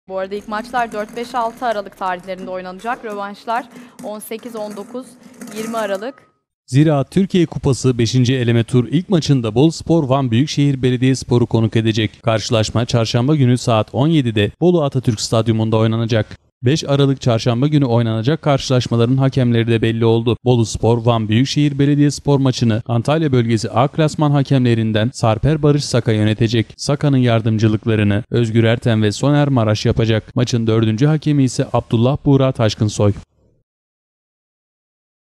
KUPADA HAKEM BELLİ OLDU (04.12.2018)
Ziraat Türkiye Kupası 5. eleme tur ilk maçında Boluspor, Van Büyükşehir Belediyespor’u konuk edecek. Karşılaşma Çarşamba günü saat 17.00’da Bolu Atatürk Stadyumunda oynanacak.